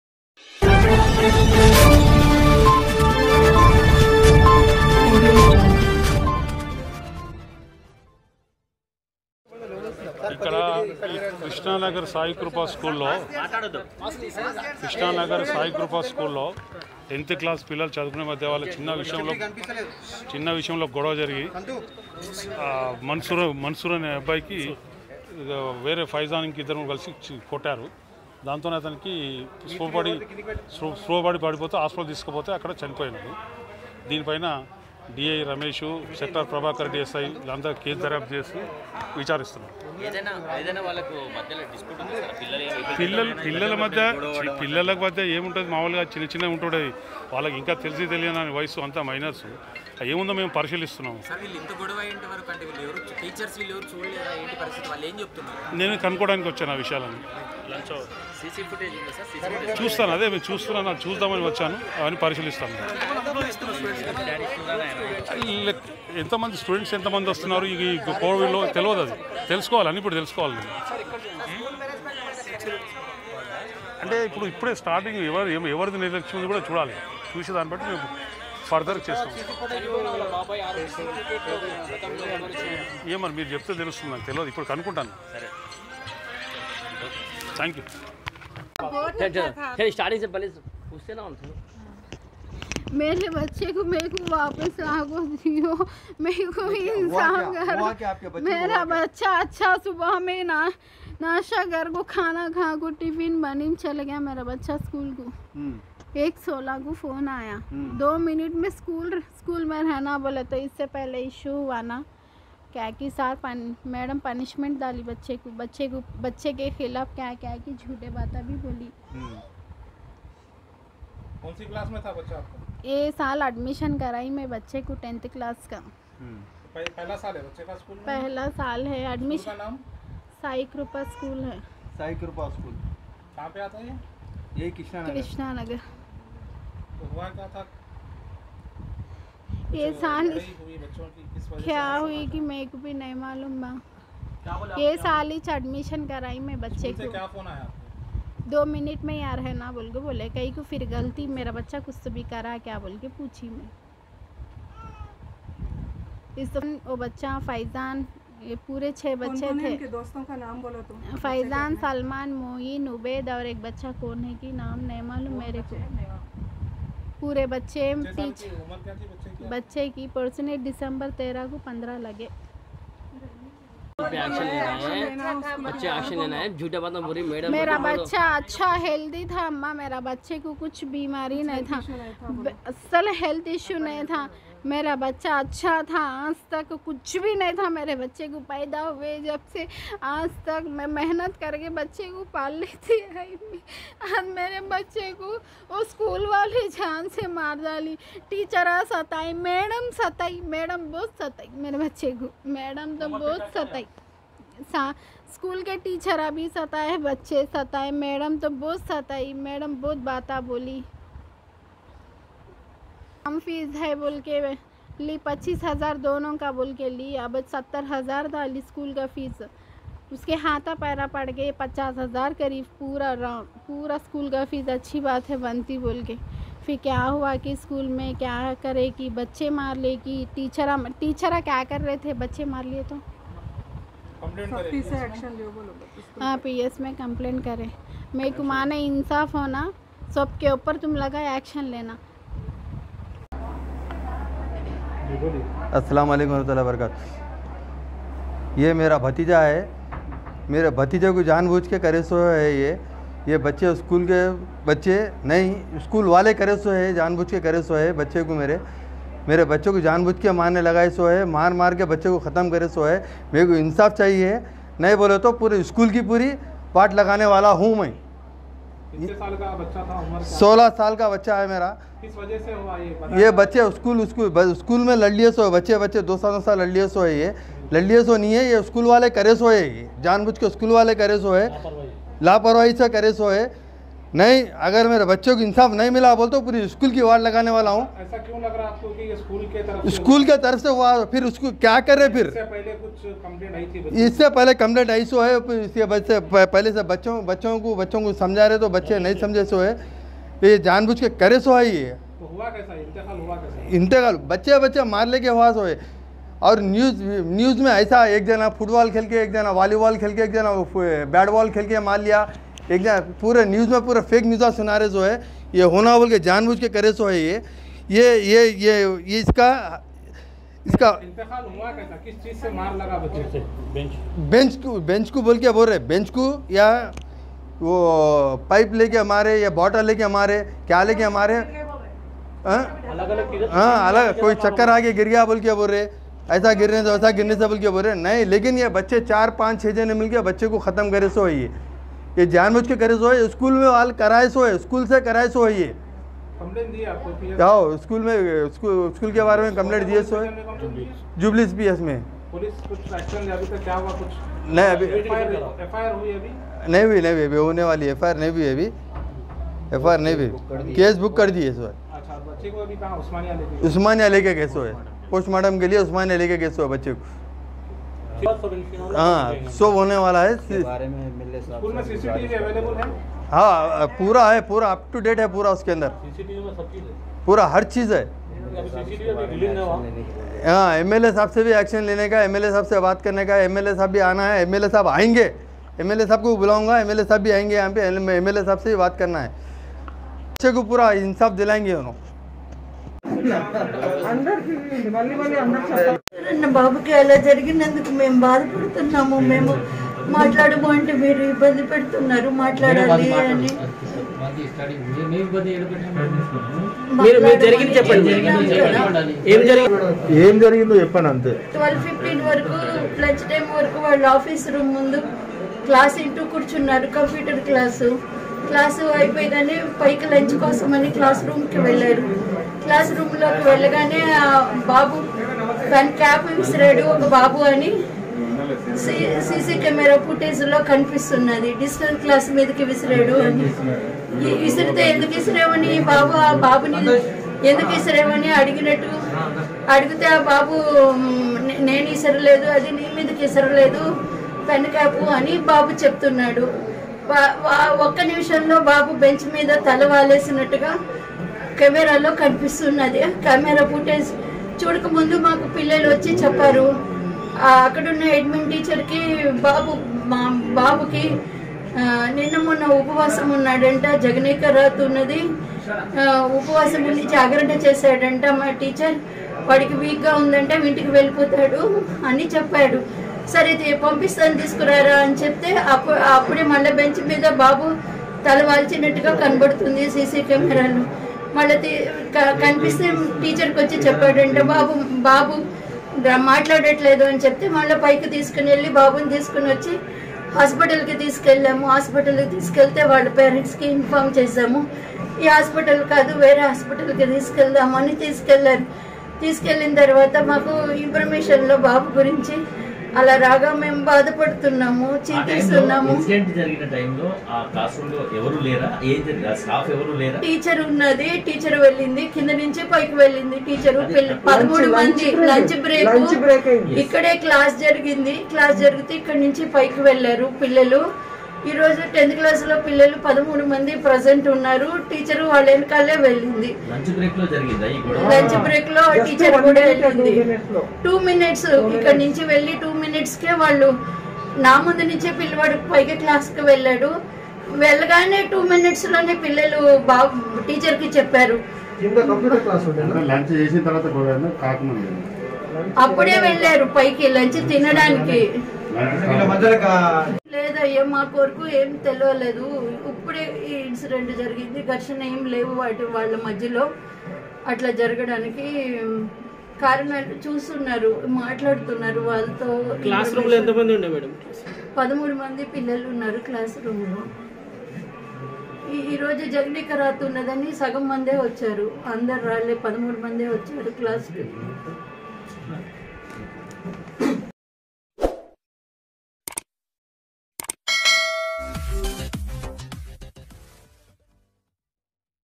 News53🖊️ School Mein Maarpeet Ek Student Ka Hua Inteqaal | Syed Manzoor Ki Maa Ka Ro Kar Bura Haal
School Mein Maarpeet Ek Student Ka Hua Inteqaal | Syed Manzoor Ki Maa Ka Ro Kar Bura Haal | 10 class Mein Padh raha tha | Sai Krupa School | Jubilee Hills Ps Limits Hyderabad